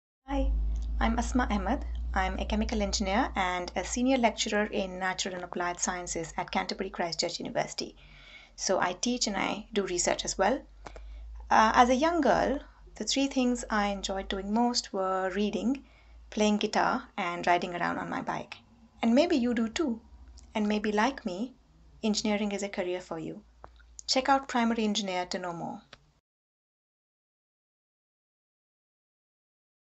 INWED21 Asma Ahmed, Chemical Engineer and Senior Lecturer in Natural Applied Sciences
To celebrate Women in Engineering Day 2021 Primary Engineer asked women engineers to create a short video to explain who they are, what they do, who they work for and three things they enjoyed most at school. To inspire young children to become the future engineers.

Meet  Asma Ahmed, Chemical Engineer and Senior Lecturer in Natural Applied Sciences at Canterbury Christ Church University.